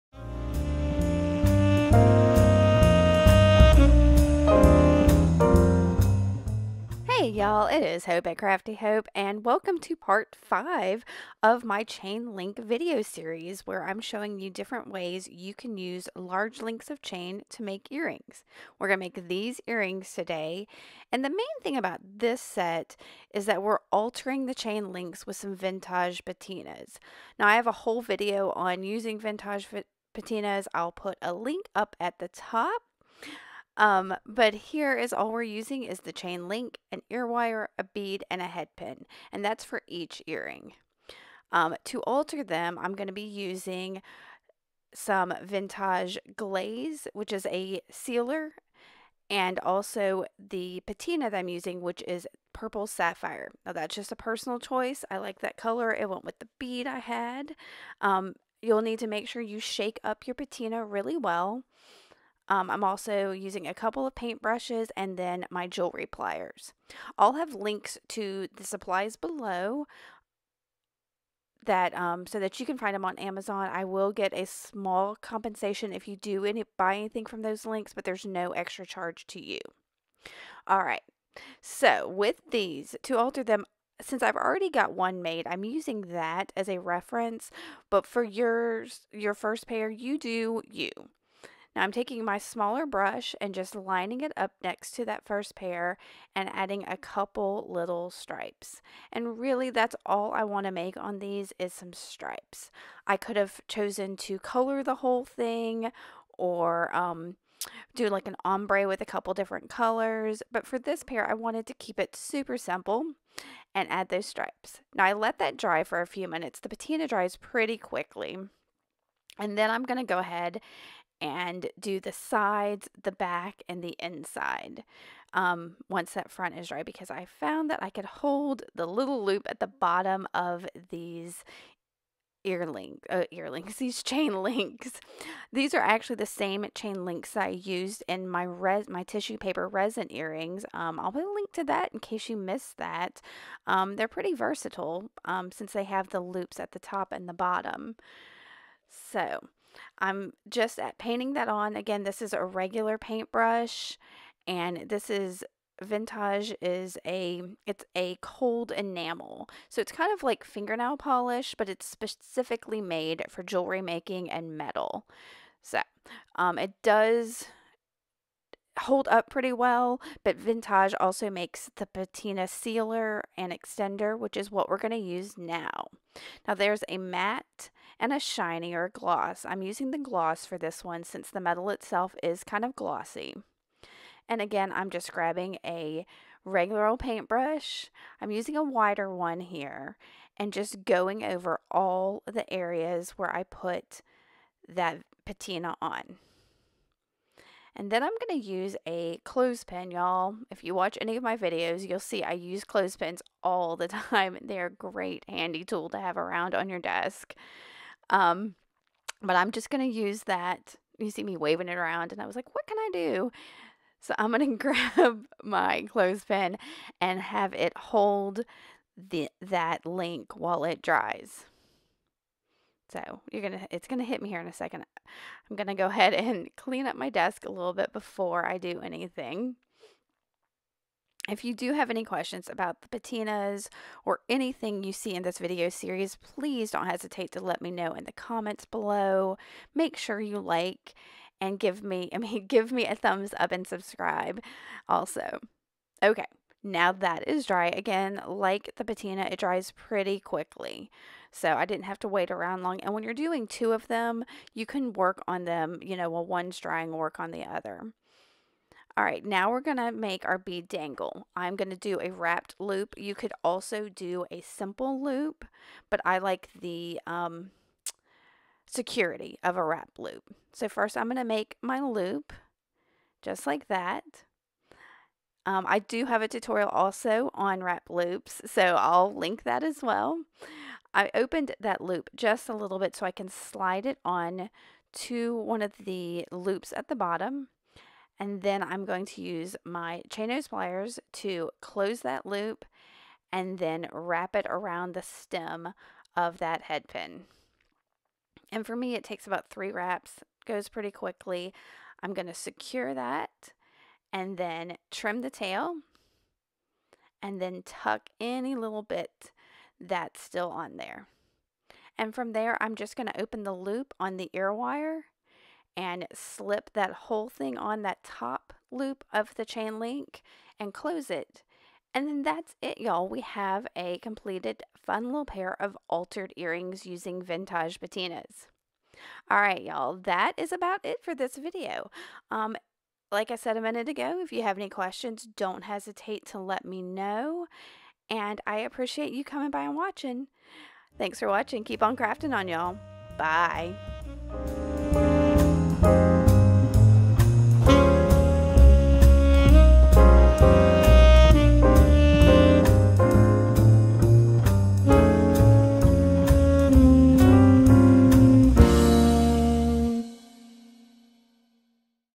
DIY Chain Link Earrings Part 5 - Altering Links with Vintaj Patina
Welcome to the Fifth video in my Chain Link Earring series in which I'm sharing ways to use large links from metal jewelry chains to make a variety of earrings. This series begins with the simplest of techniques and materials and will work up to harder and more complex ones. If you missed my first four videos, I’ve created a playlist

In this video, I use Vintaj Patina (from Ranger Ink) to alter the links to match the beads I add to them. If you're looking for other ways to use the Patinas, I have another video that might interest you

Materials*
Vintaj Patina patina&index=aps&camp=1789&creative=9325&linkCode=xm2&linkId=632c6e36e795fac755ce52fd0588f926
Wubbers Pliers
Flush Cutters
Head Pin assortment
Ear Wire Assortment
Vintaj Glaze

If you have any questions about my process, materials, or anything else; please don't hesitate to ask!